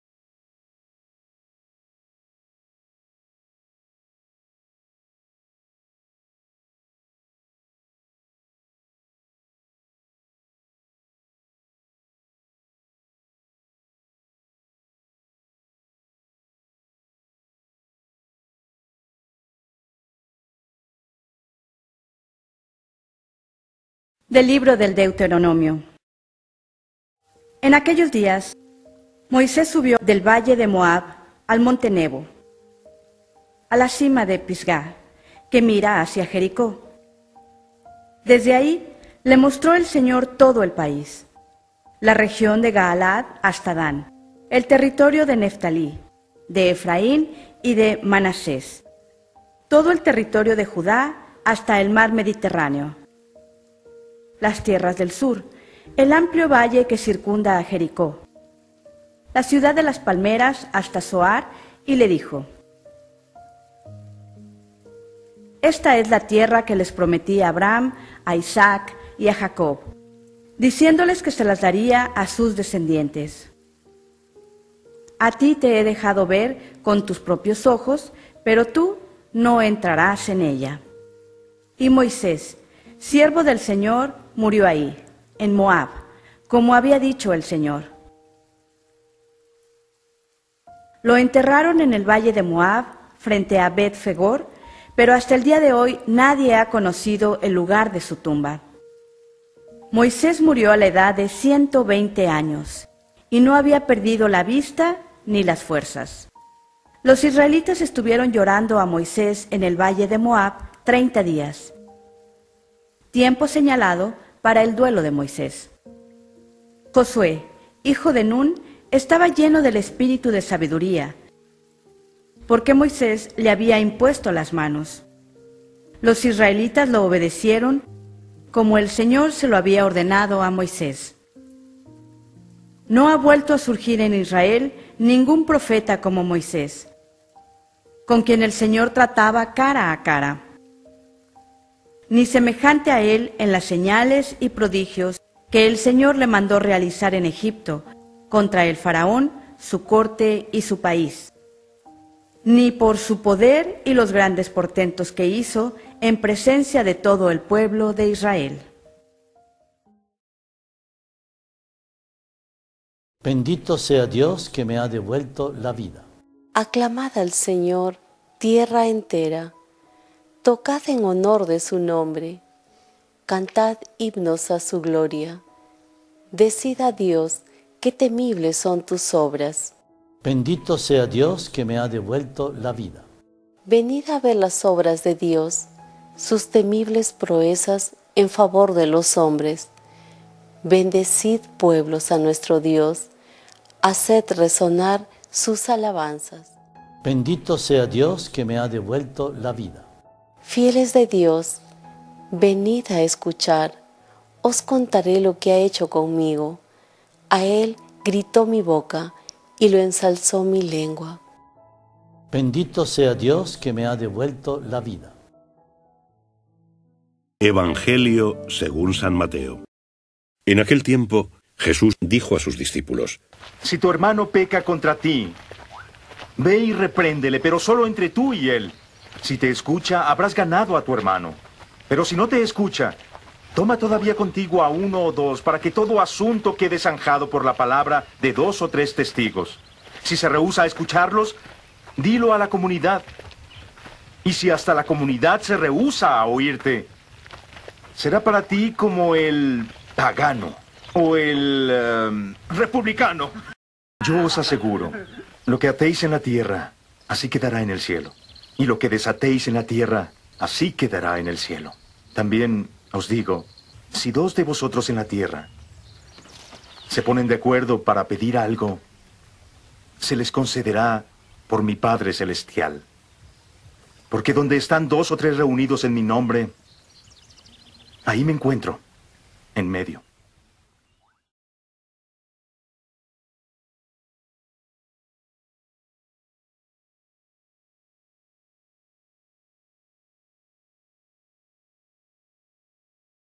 LECTURAS Y EVANGELIO MIÉRCOLES 16 DE AGOSTO DE 2023 | MIÉRCOLES XIX T.ORDINARIO | MATEO 18,15-20
EVANGELIO Y LECTURAS EUCARISTÍA MIÉRCOLES 16 DE AGOSTO DE 2023 MIÉRCOLES DÉCIMA NOVENA SEMANA DEL TIEMPO ORDINARIO Primera Lectura: Del libro del Deuteronomio 34,1-12 Salmo 65,1-3a.5.8.16-17 R/. Bendito sea Dios, que me ha devuelto la vida Evangelio según San Mateo 18,15-20